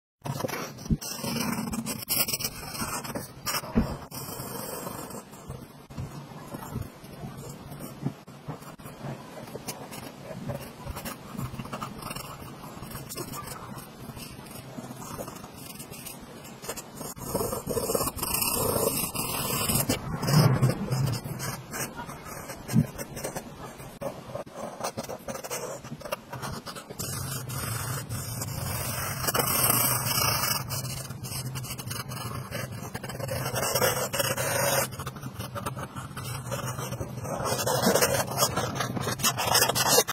drawing drawing drawingideas artshorts foryou youtubeshorts
Explore the artistry of Nurul Islam Ador in this captivating painting tutorial video. Join me as I bring to life a beautiful girl through the strokes of my brush. Discover the techniques and inspiration behind creating a stunning portrait. Whether you're a seasoned artist or just starting, this video is sure to inspire your next masterpiece. Dive into the world of art with this step-by-step guide to painting a girl by Nurul Islam Ador. 🎨